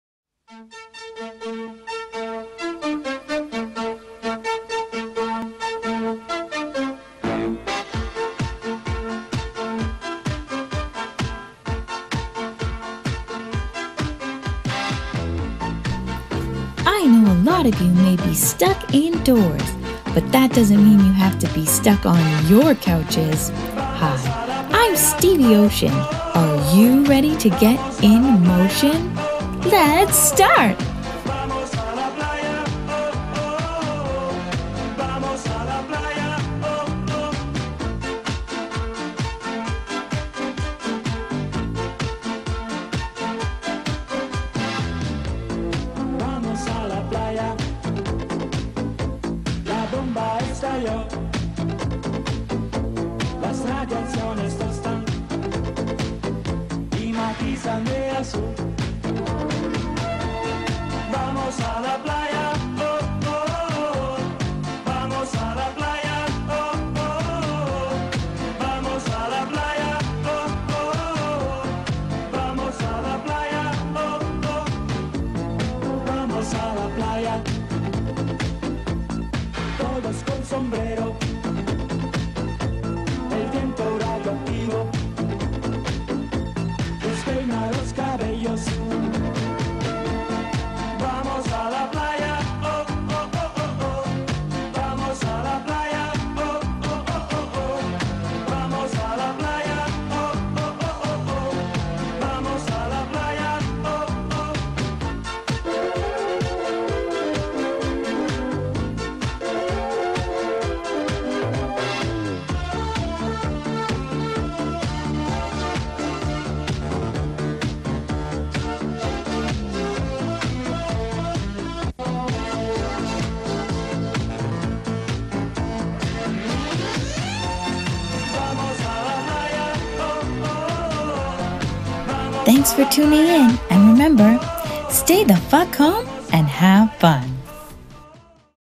Motion with Stevie Ocean
In isolation? Gym closed? Use this work out video to stay fit while quarantined.